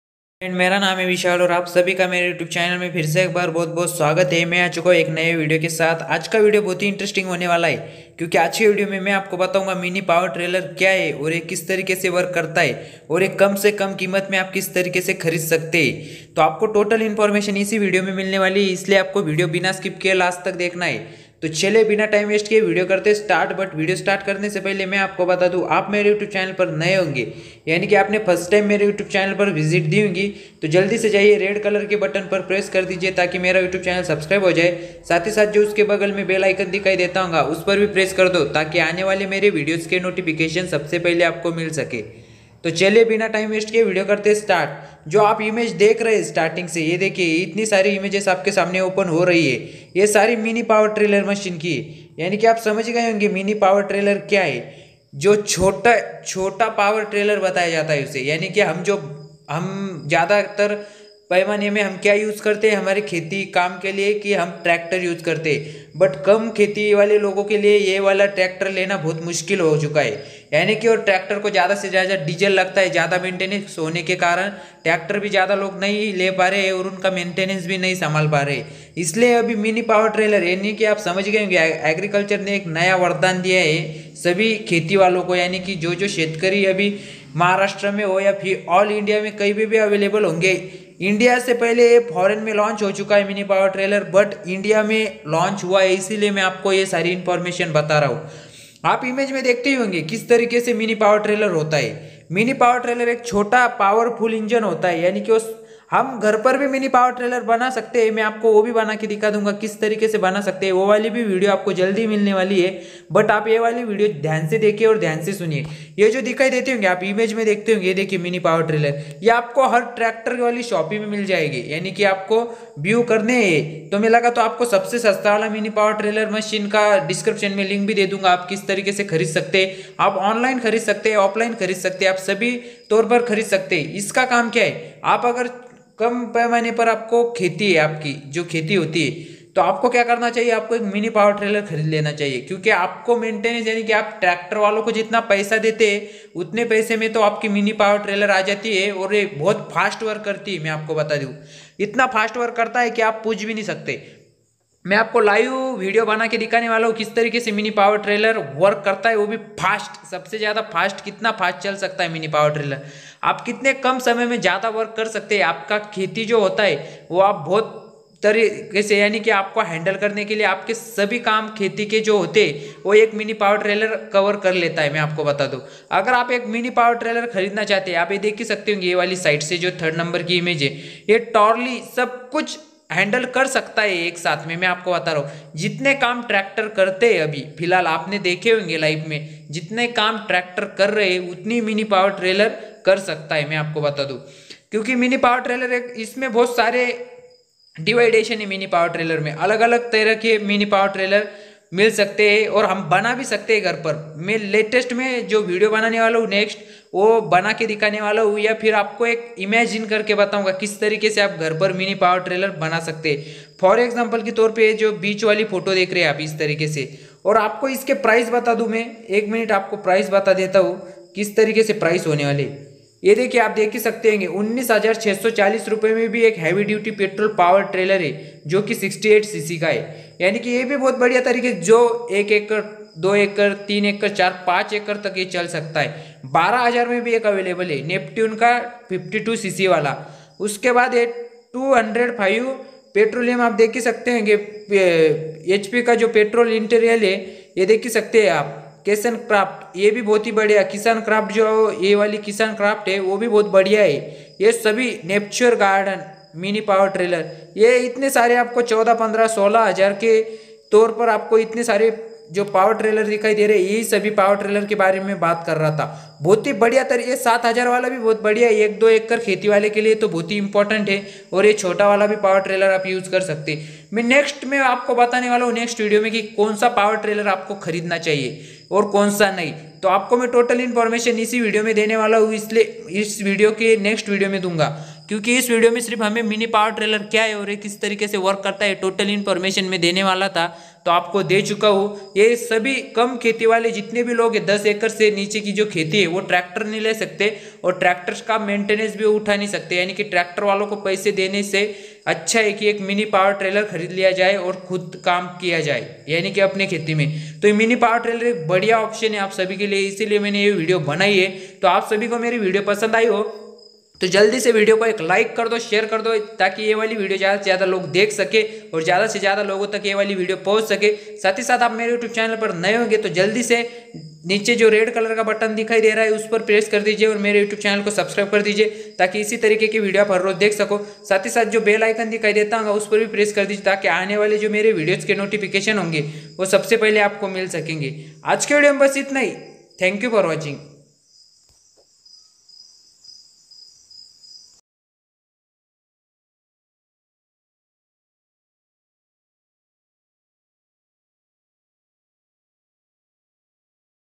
Mini Power Trailer | Full explain of mini power trailer 2019
Hello Freiend welcome to the new video

Aaj ke video me mene mini power trailer full details di he agar video pasand aaya to Mere video ko like share or mere channel ko subscribe Karna na bhule thankyu for watching